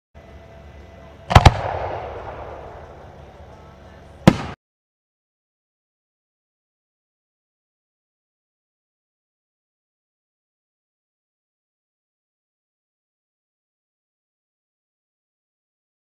Freedom 4" white wave to special orange strobe
2021 Casabella Demo

sound on this video was muted because music playing in the background had copyright violation